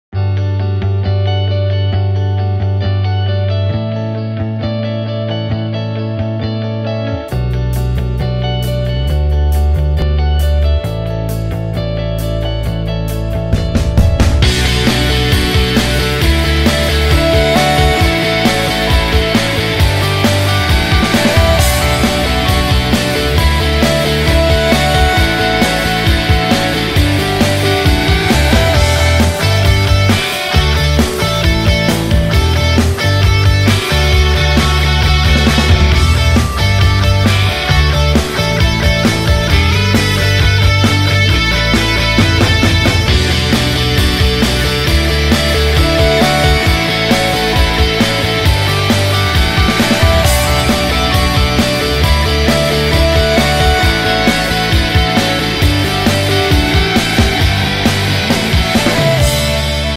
DJI Spark and the stunning Salton Sea
The rain has come to So Cal and the Salton Sea is no longer a pool of death.  The water levels have risen enough to cover the masses of dead Corvina leaving us with a picturesque paradise without the smell of dead fish!